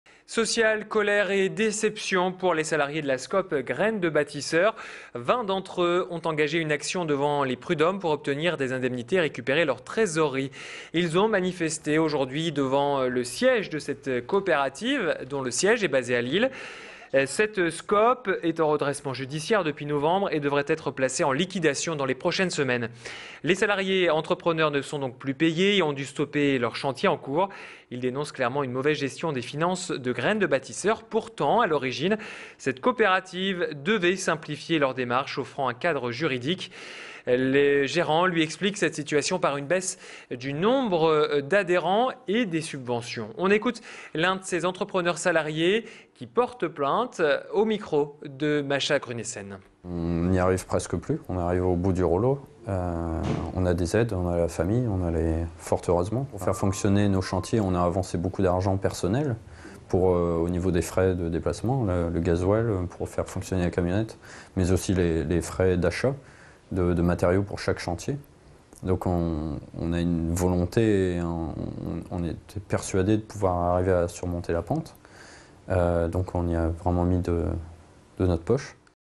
Graines de Bâtisseurs : Vers la liquidation ! (Lille)
La SCOP Graine de Batisseurs devrait malheureusement être placée en liquidation judiciaire

Sites webs/blogs, copiez cette vidéo avec le bouton "Intégrer" !